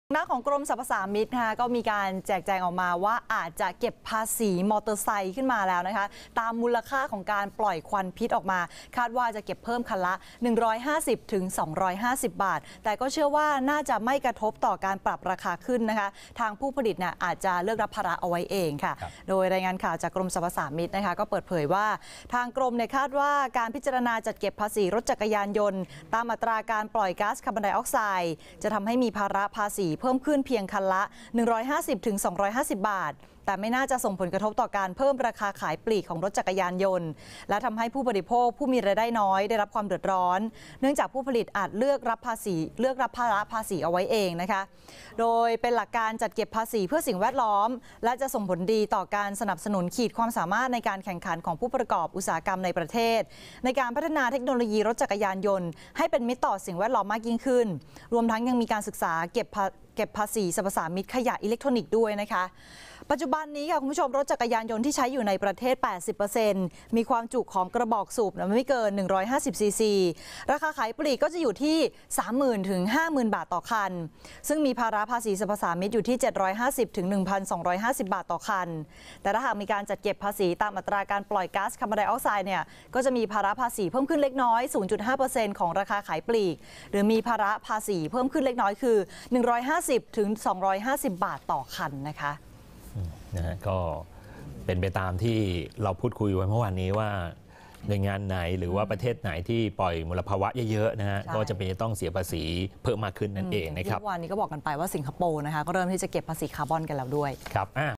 กรมสรรพสามิตเตรียมเก็บภาษีจักรยานยนต์ ตามอัตราการปล่อยคาร์บอนฯ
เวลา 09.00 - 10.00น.